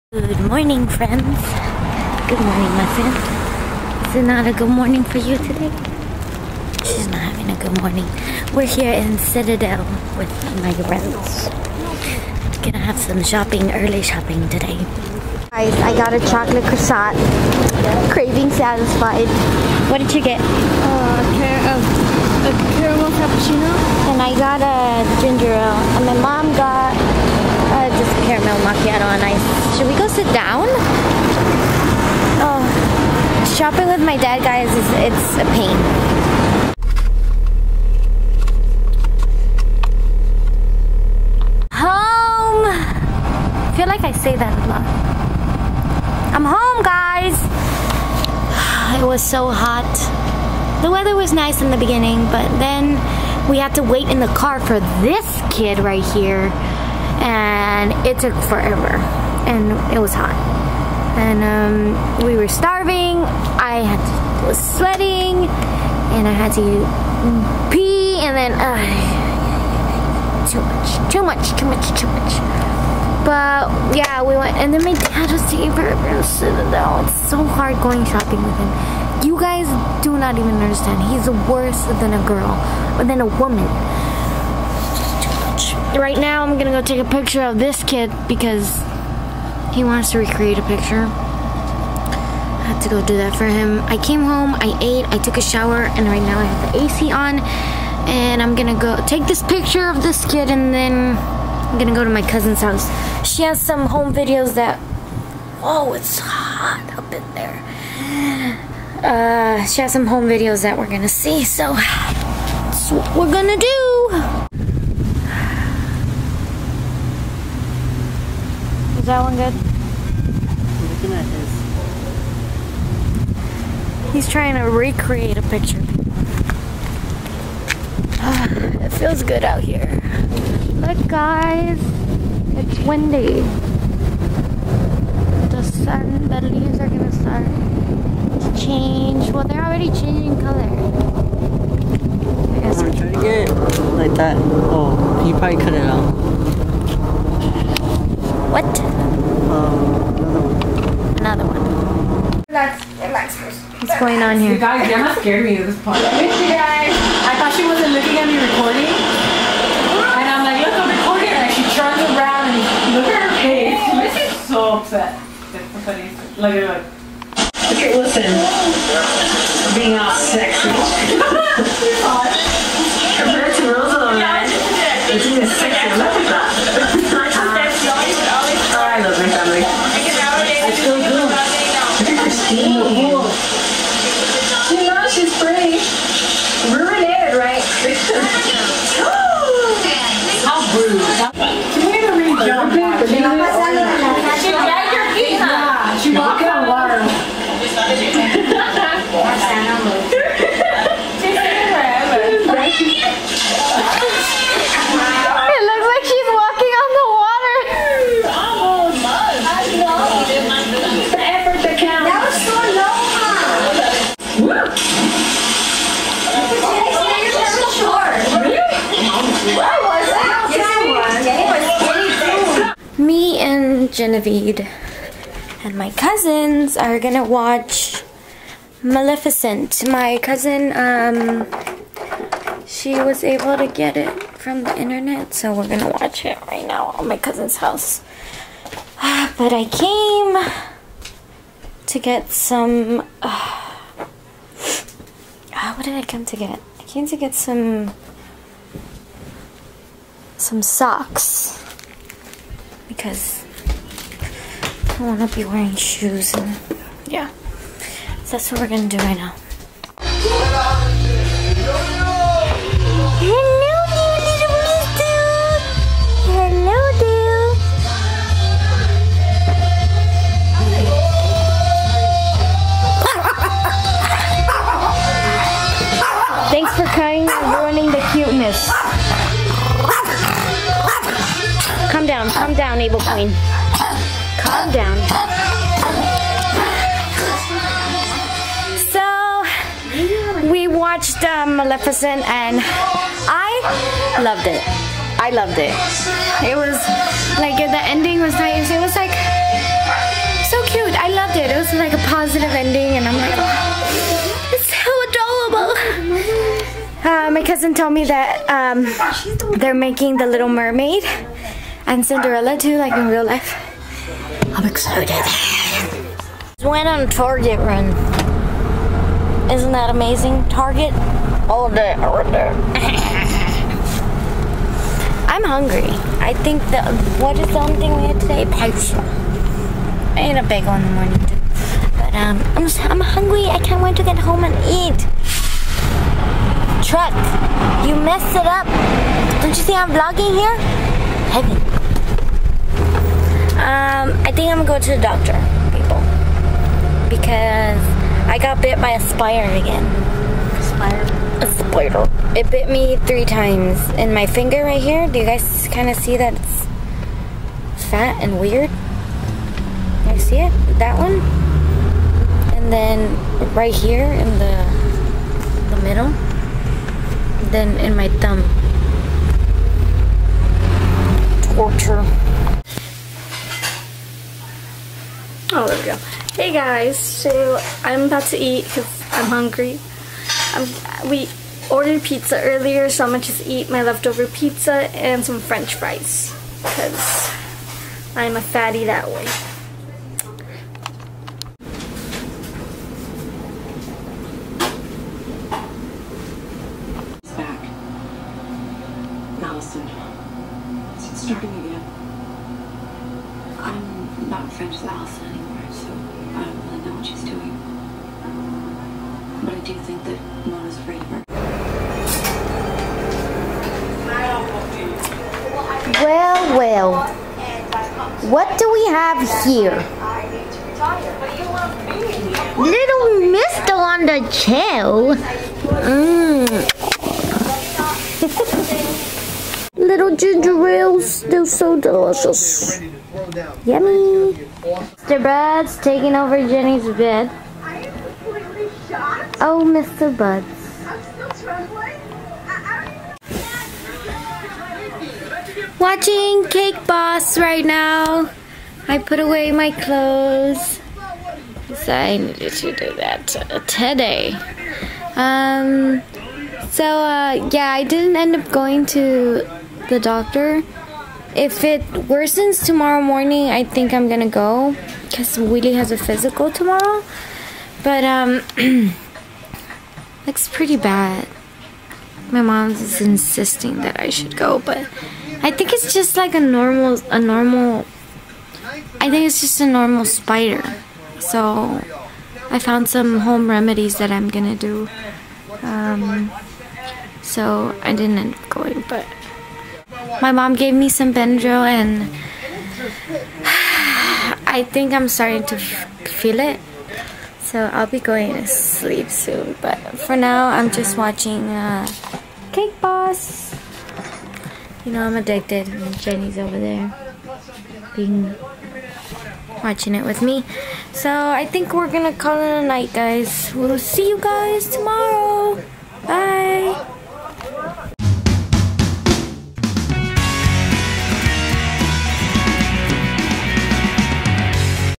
Home Videos - August 26, 2014
Hey Guys :)
My siblings and I are trying out this vlogging our day thing. Its just for fun! Hope you guys enjoy a little dose of our lives!